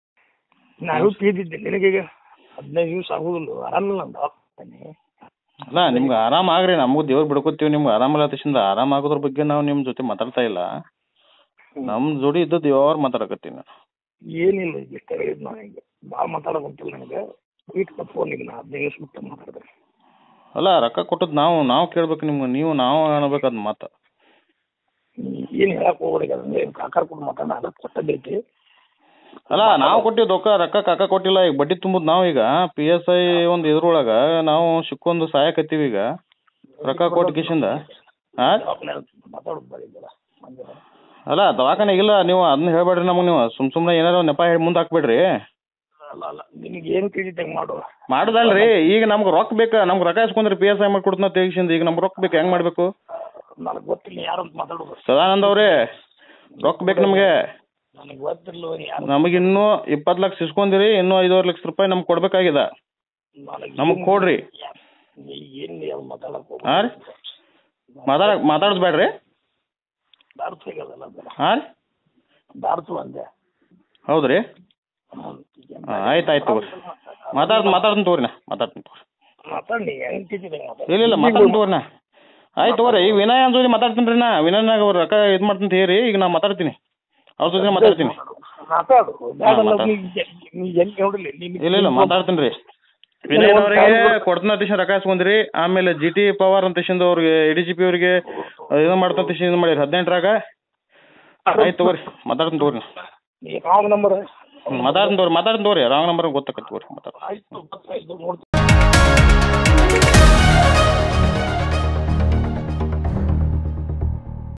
PSI || PSI ನೇಮಕಾತಿ ಬಗೆದಷ್ಟೂ ಅಕ್ರಮ ಬಯಲು || Zee Kannada News
PSI ಅಕ್ರಮ ನೇಮಕಾತಿ ಸಂಬಂಧ ಸ್ಫೋಟಕ ಆಡಿಯೋ ಬಿಡುಗಡೆಯಾಗಿದೆ. ಈ ಆಡಿಯೋದಲ್ಲಿ ದುಡ್ಡು ಕೊಟ್ಟಿರೋ ಅಭ್ಯರ್ಥಿಯೊಬ್ಬ ಮಧ್ಯವರ್ತಿ ಬಳಿ ವಾಪಸ್‌ ಕೇಳಿದ್ದಾನೆ. ಅಲ್ದೆ ಸದ್ಯಕ್ಕೆ ಸುಮ್ಮನಿರು. ಆಮೇಲೆ ಎಲ್ಲವೂ ಸರಿಯಾಗುತ್ತೆ ಅಂತಲೂ ಸಮಜಾಯಿಷಿ ಕೊಟ್ಟಿರೋ ದಲ್ಲಾಳಿಯ exclusive ಆಡಿಯೋ ನಿಮ್ಮ ಜೀ ಕನ್ನಡ ನ್ಯೂಸ್‌ನಲ್ಲಿ..